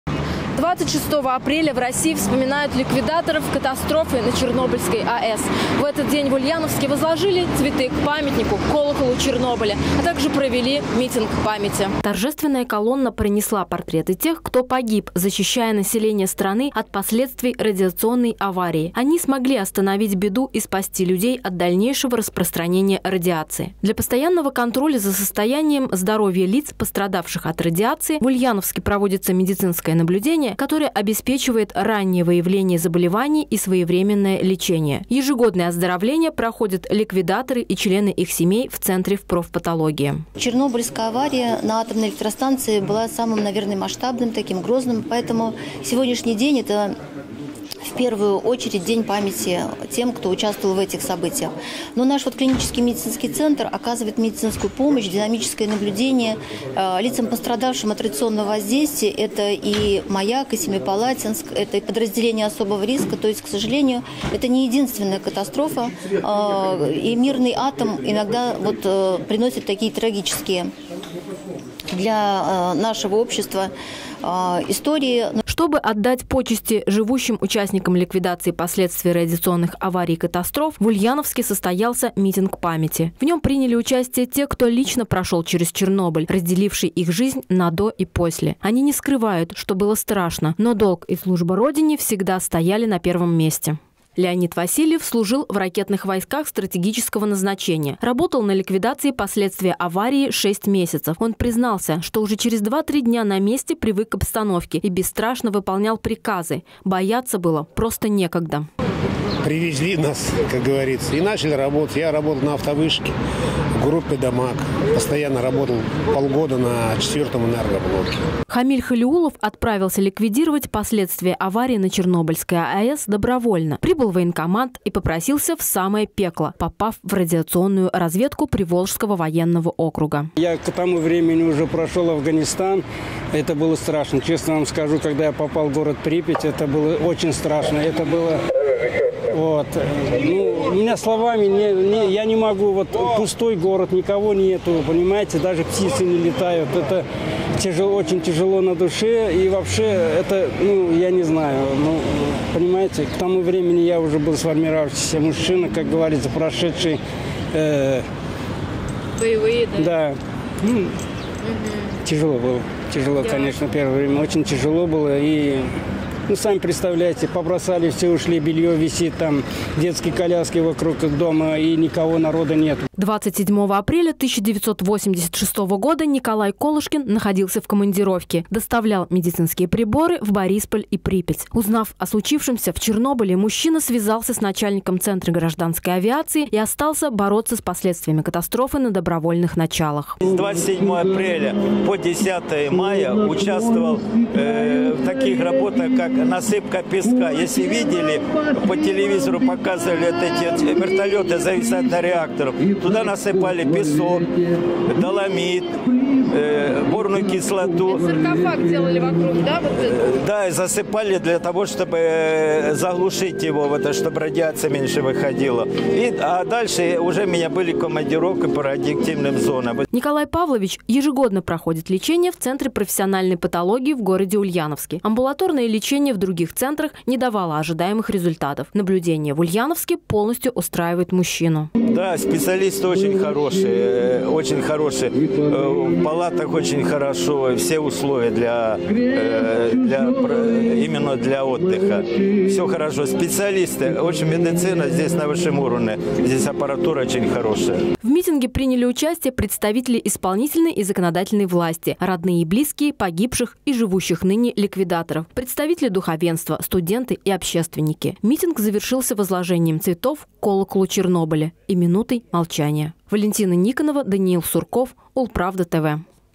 В Ульяновской области почтили память жертв и ликвидаторов Чернобыльской катастрофы
26 апреля в России вспоминают ликвидаторов катастрофы на Чернобыльской АЭС. В этот день в Ульяновске возложили цветы к памятнику – Колоколу Чернобыля, а также провели митинг памяти.